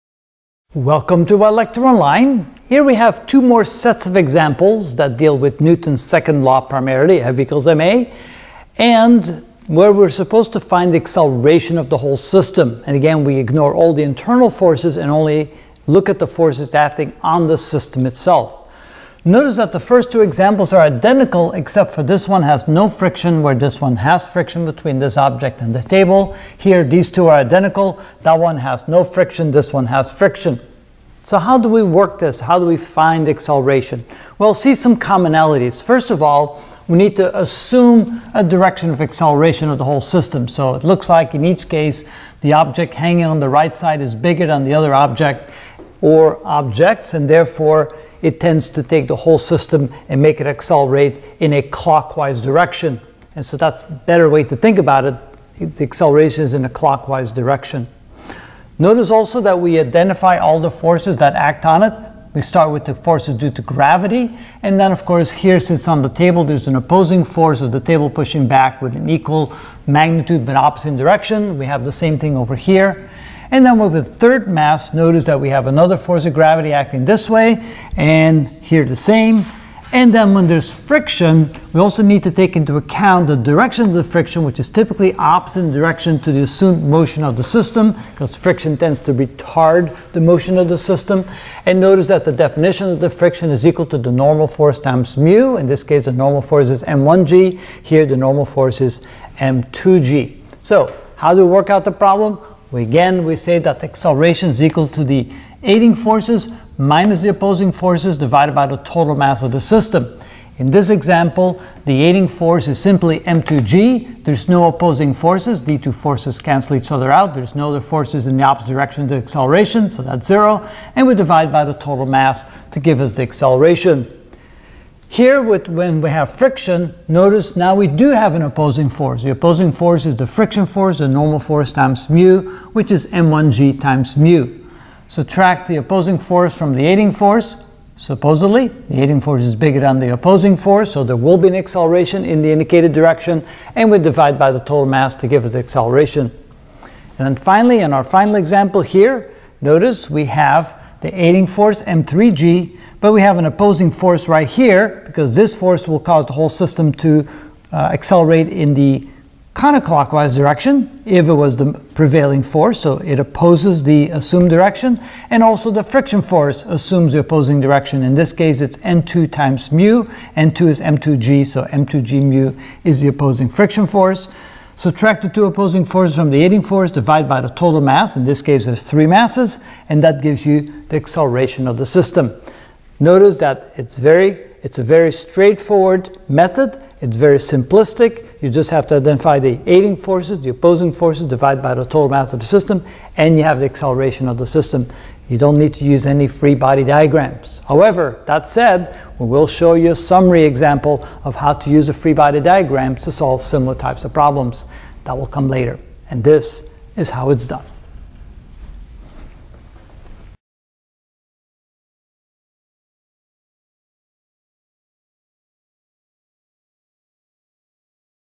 Physics Review: Applications of Newton's 2nd Law (a=?) (Part 2 of 3)
(Part 2) We will learn how to review and study for a test so we can quickly solve a=?, acceleration of the system, of some typical variations of a system of 2 or 3 boxes connected to each other on a table top or hanging off a table top using Applications of Newton’s Law, F=ma.

Title seach on other plateforms:  Physics Review: Applications of Newton's 2nd Law (a=?) (Part 1)

Title search on other plateforms:  Physics Review: Applications of Newton's 2nd Law (a=?) (Part 3)